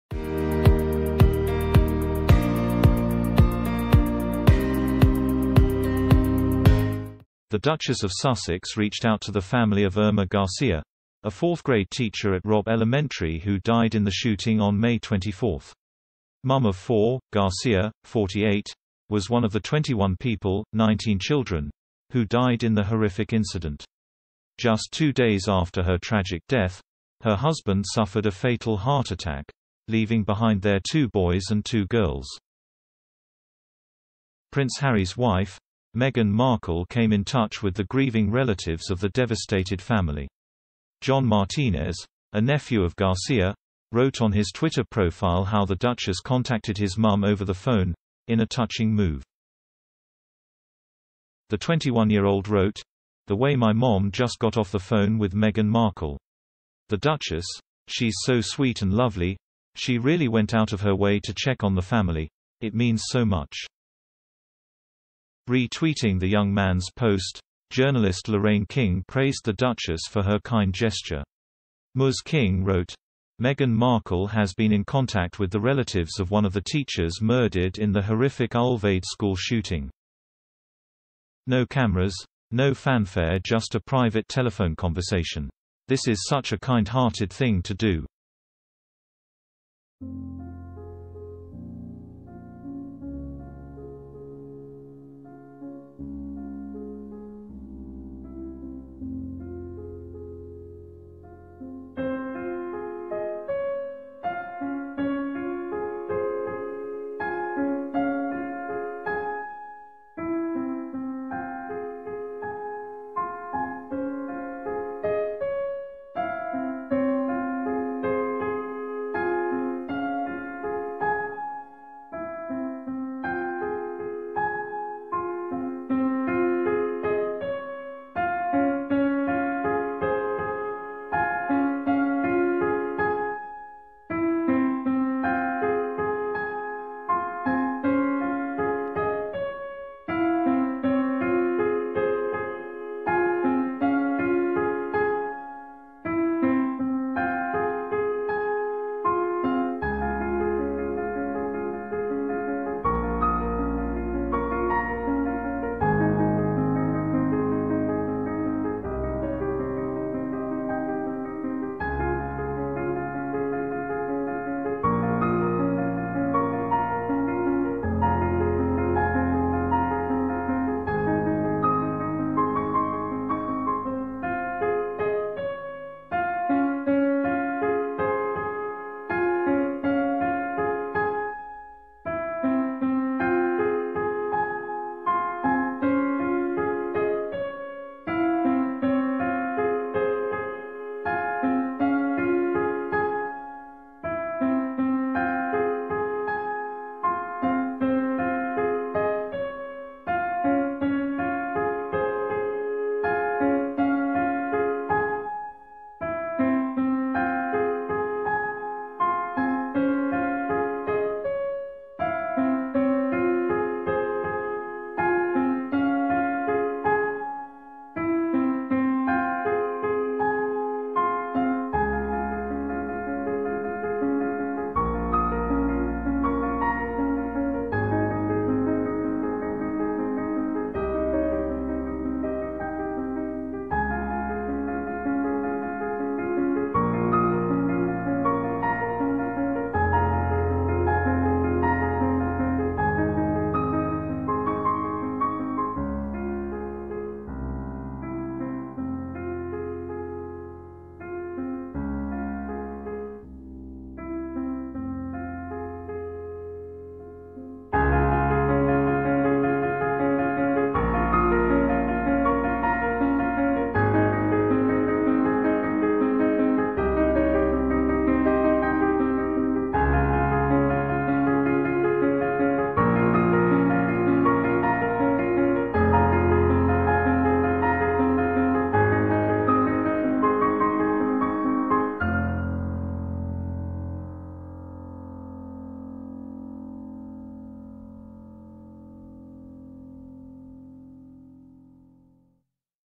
'No cameras, no fanfare' Meghan Markle praised for humble new intervention
John Martinez, a nephew of Garcia, wrote on his Twitter profile how the Duchess contacted his mum over the phone, in a touching move.

The 21-year-old wrote: “The way my mom just got off the phone with Meghan Markle…

“The Duchess, she’s so sweet and lovely, she really went out of her way to check on the family, it means so much.”